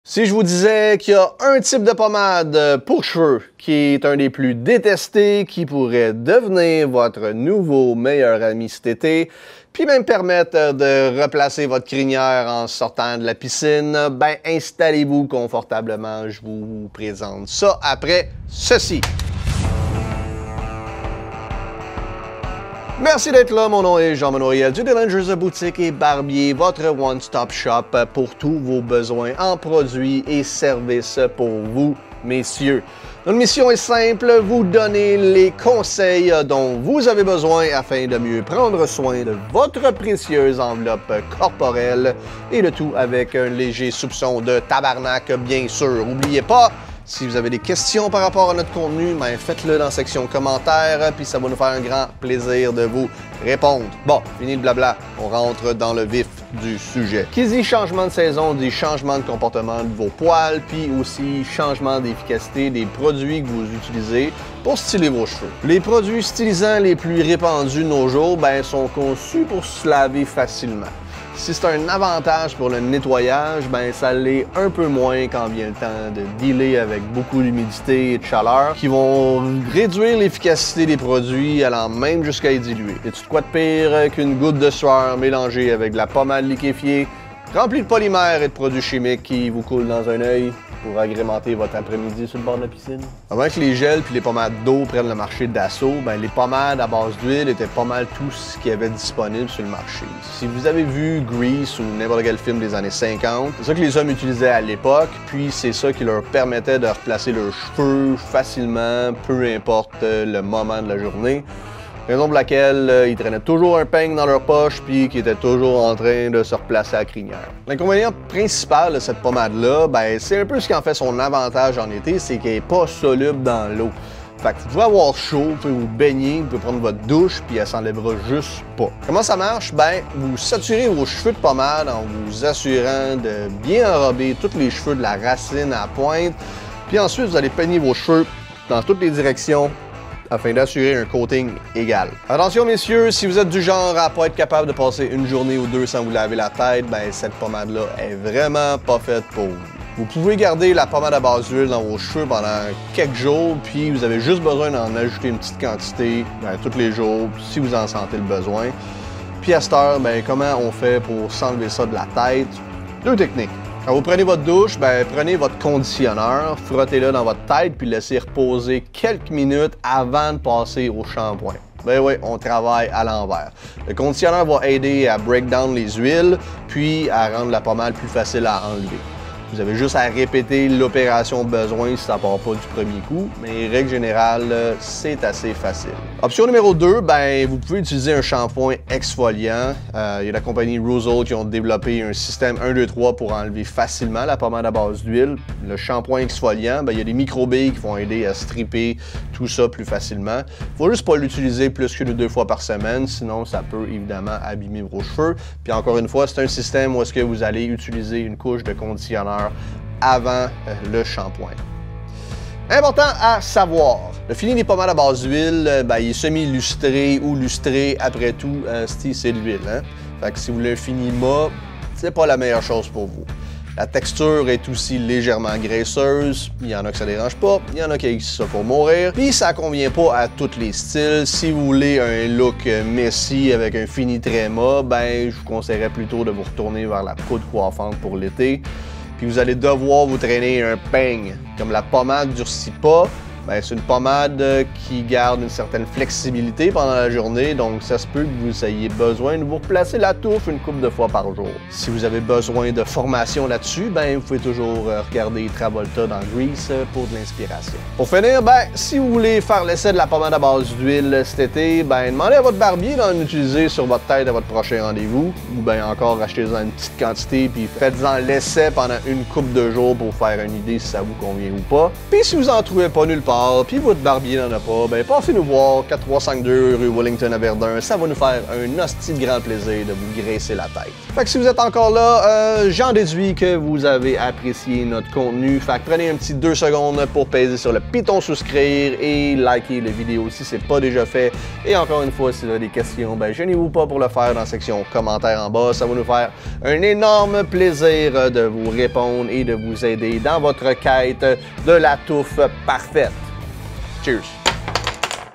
Meilleure cire coiffante pour l'été? La pommade la plus détestée pourrait être la solution!
La pommade à base d'huile est souvent détesté par sa texture et du au fait qu'elle est difficile à laver. Pourtant. qu'est-ce que tu dirais de pouvoir te replacer la touffe en sortant de la piscine ou de la douche?

La majorité des pommades et des gels sont à base d'eau afin d'être plus facile à laver...cependant, avec la chaleur...ils fondent, ruinent votre style et se mélange à la sueur pour mieux vous brûler les yeux!

On t'explique comment faire pour que la pommade à base d'huile devienne ta meilleure amie cette été!

Pour plus d'info sur nos produits, passez nous voir au Dillinger's boutique et barbier au 4352 rue Wellington Verdun Qc H4G-1W4